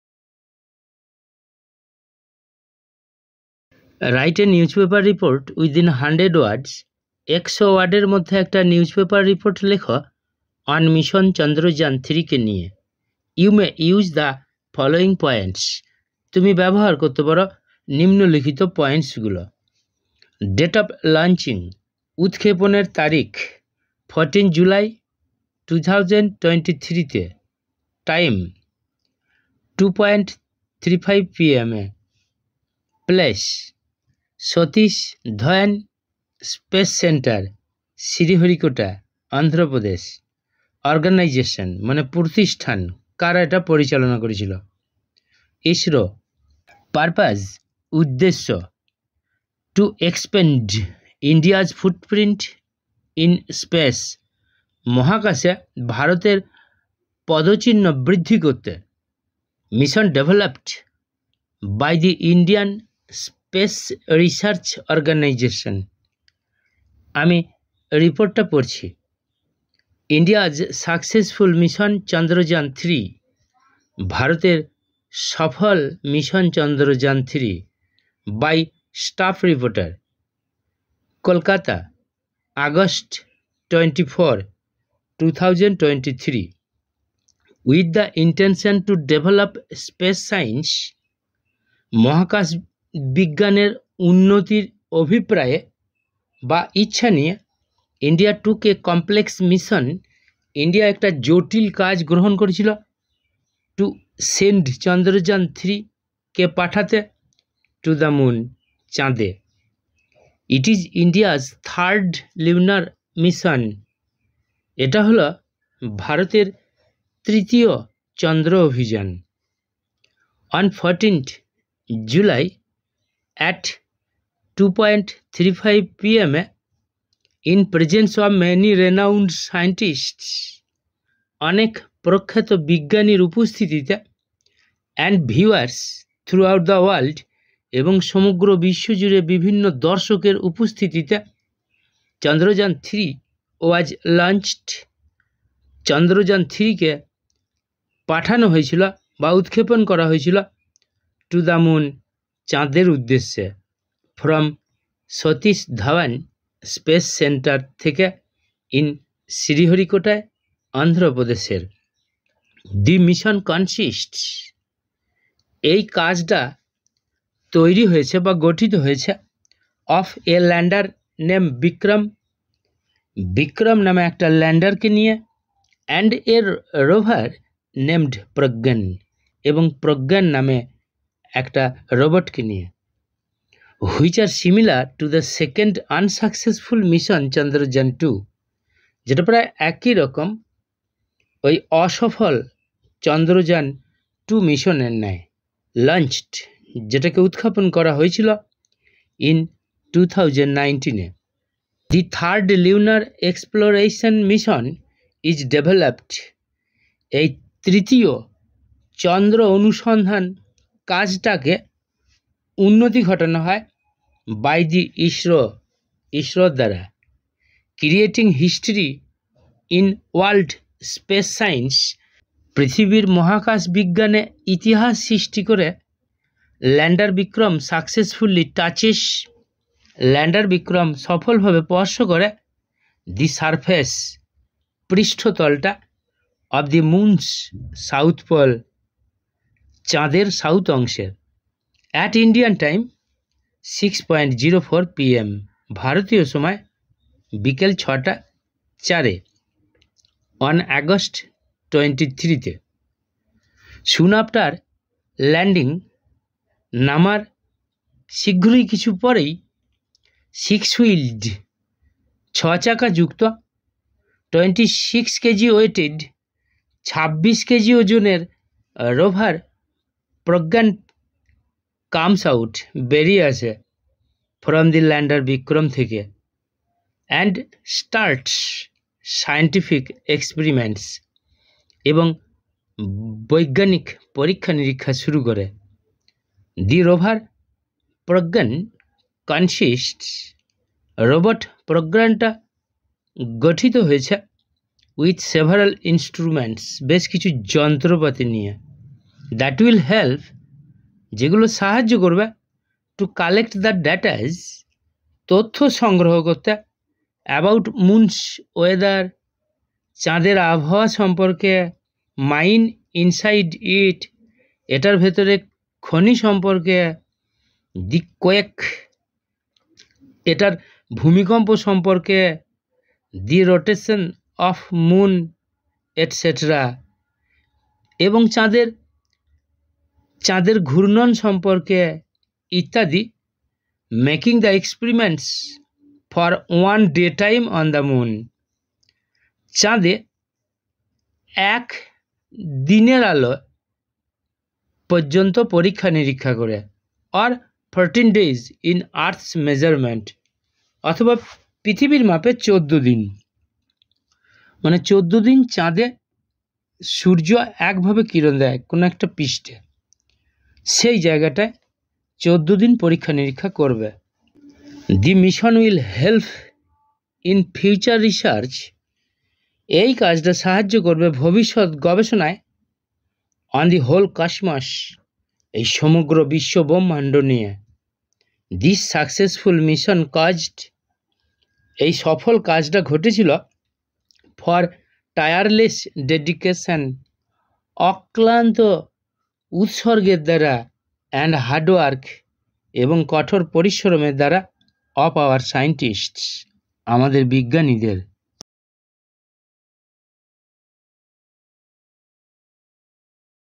India's Successful Mission Chandrayaan-3 / Report Writing for students / Chandrayaan 3 / VIDEO 1
Write a newspaper report (within 100 words) on "Mission Chandrayaan-3". You may use the following points:
[[ Date of launching : 14 July, 2023,
Time : 2.35 p.m,
Place : Satish Dhawan Space Centre, Sriharikota (শ্রীহরিকোটা), Andhra Pradesh
Organisation : ISRO,
Purpose : help to expand India's footprint in space -- mission developed by the Indian Space Research Organisation ]]

বাকি VIDEO গুলি POST করা হচ্ছে - -